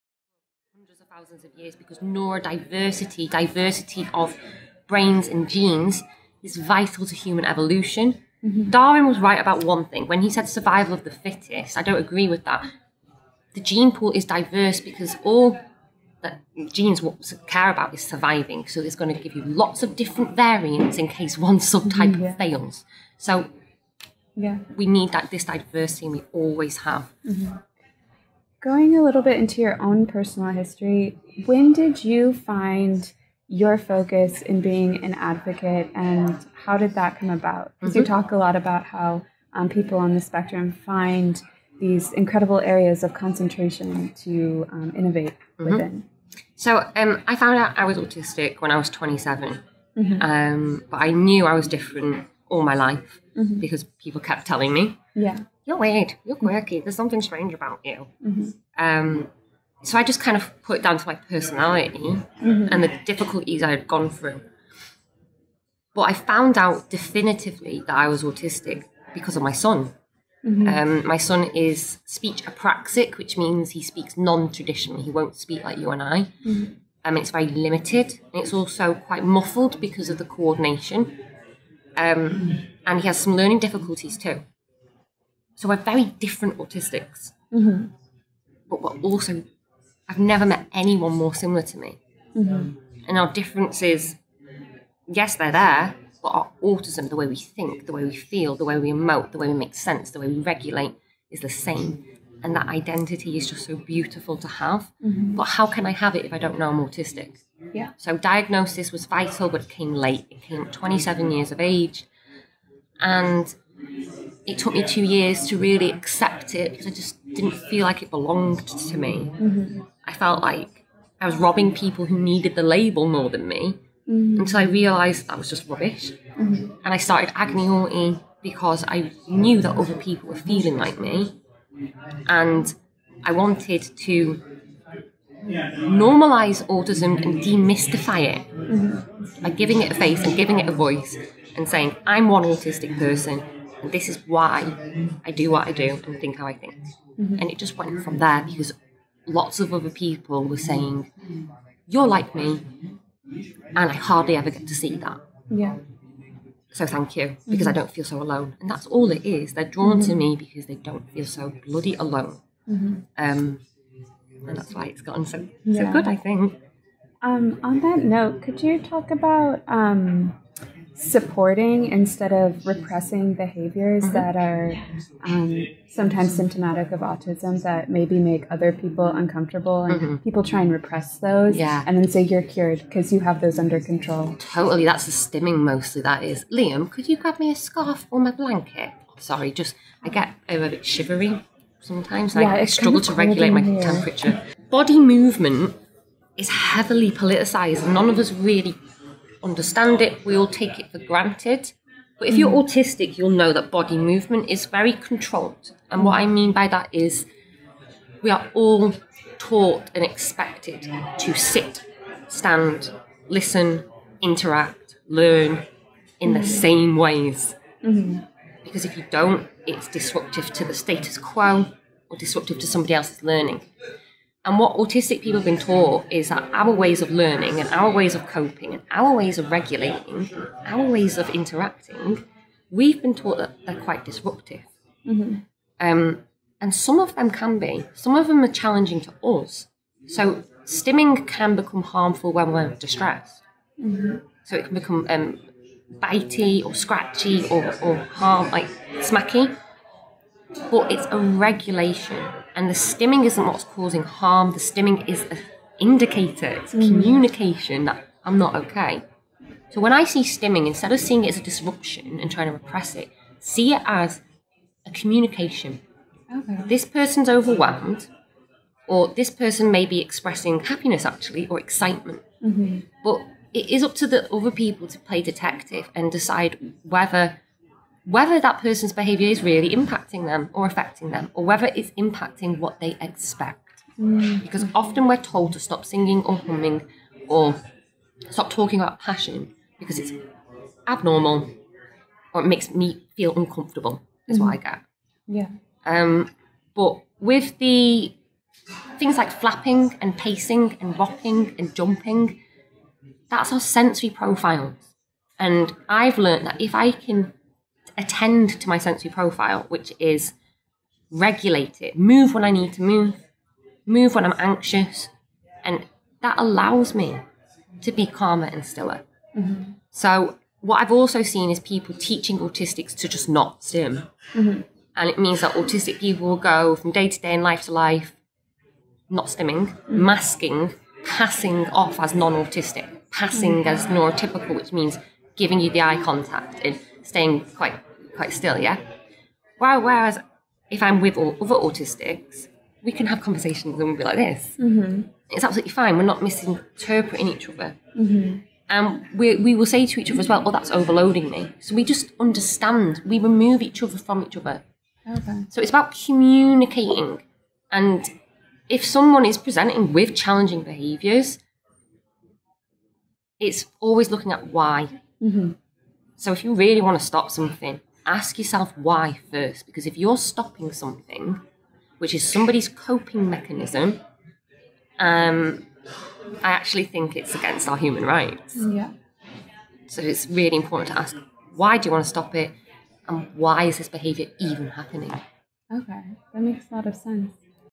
Neurodiversity and Autism | SmartRecruiters Interview
At Hiring Sucess 19, Sara was interviewed by Kaya Payseno, Sr. Editorial Manager at SmartRecruiters, about how Agony Autie came to be, and how body movement is heavily politicised.

Autistic stimming is discussed and how verbal communication is not the only way to communicate.

My son is Speech apraxic and uses echolia and other means of communicating. I’m limited to express myself in the spoken word and too much focus is placed on this ability.

It makes autistics with speech & communication difficulties/differences, vulnerable in the education and workplace.